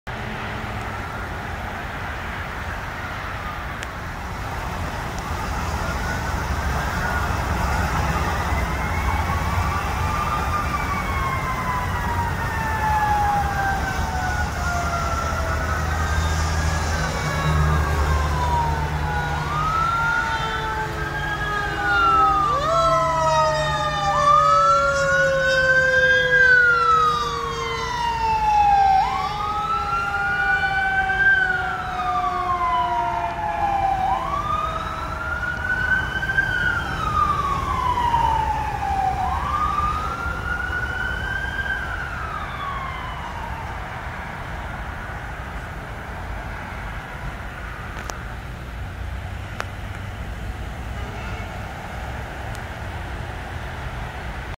Orange County Fire Rescue Unknown Engine and Medic 1 responding code 3.
OCFRD Engine and Medic 1 responding to unknown call code 3.  Enjoy.